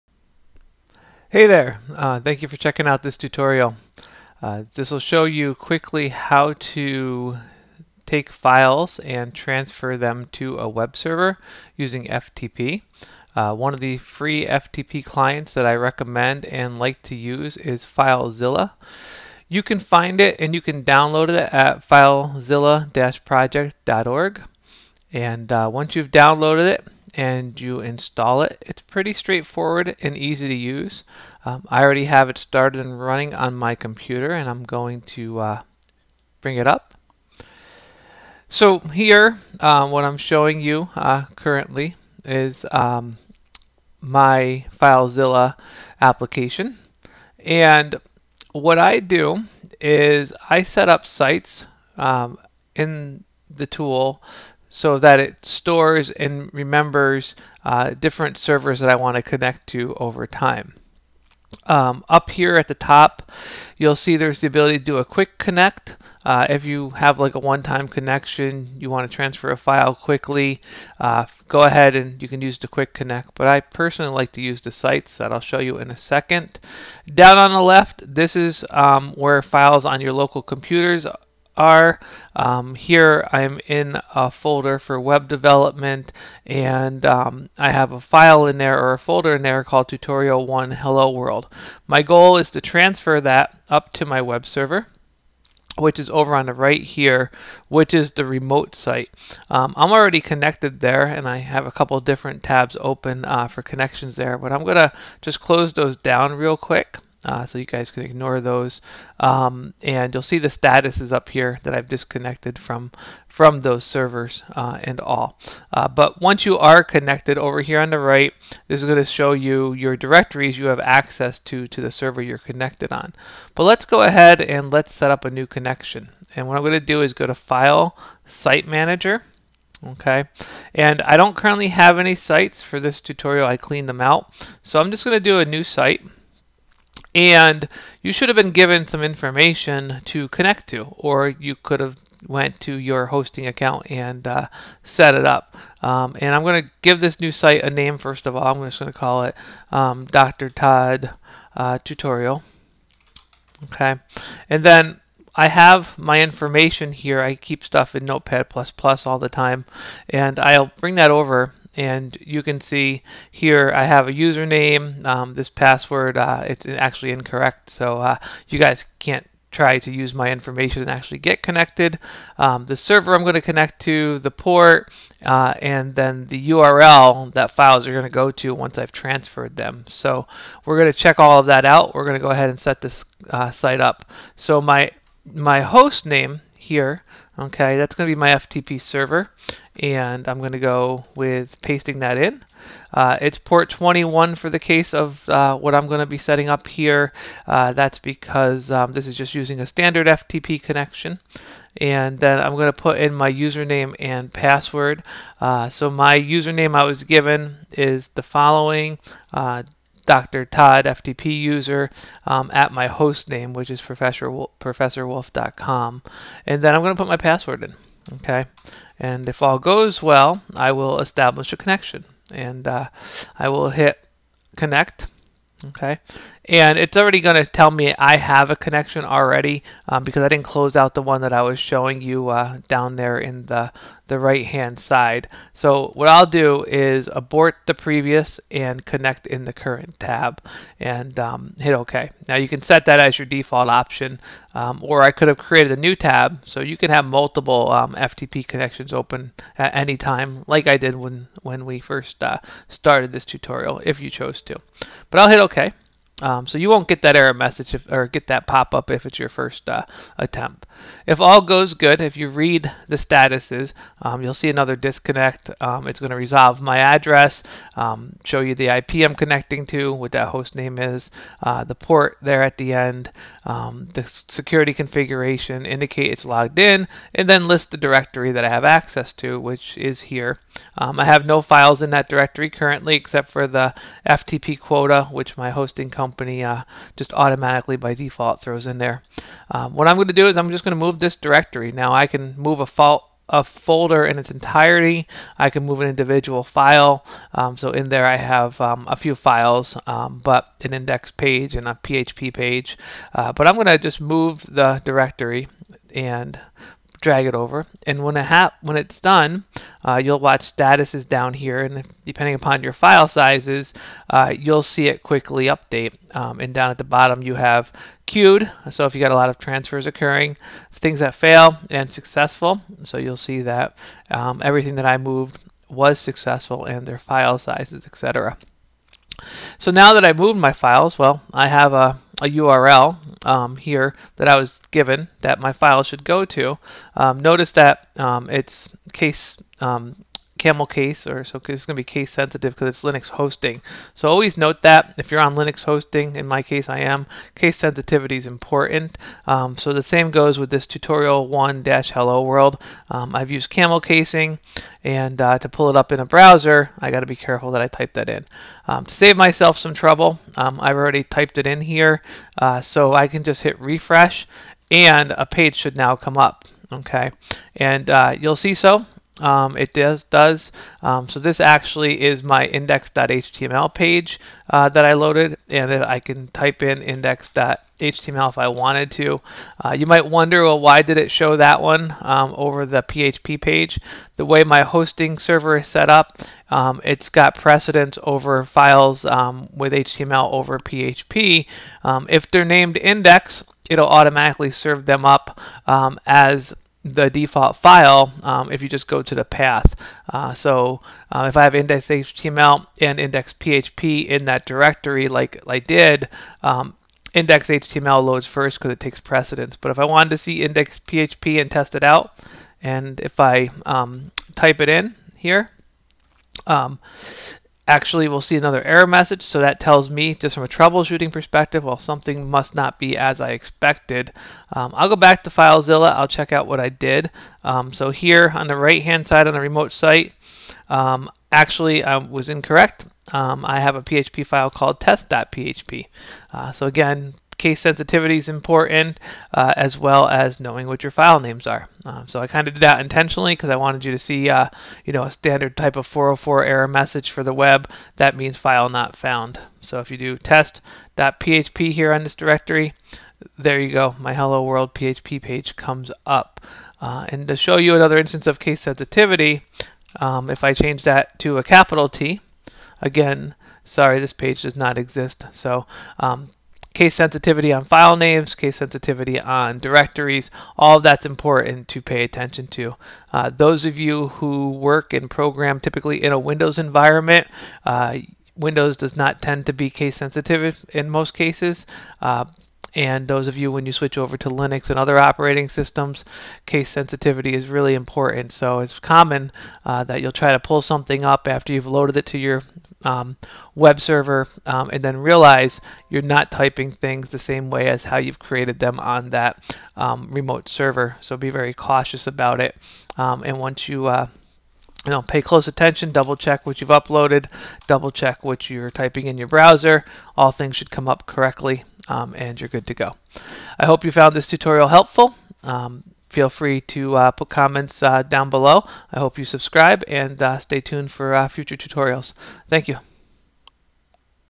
FileZilla Tutorial: How to connect to FTP and transfer files to a web server
In this tutorial, Dr. Wolfe demonstrates how to use FileZilla to connect to a web server via FTP. Once connected Dr. Wolfe demonstrates how to transfer a folder with one file and then verify that the web page transferred is available via a public URL. At the end of the tutorial you will know how to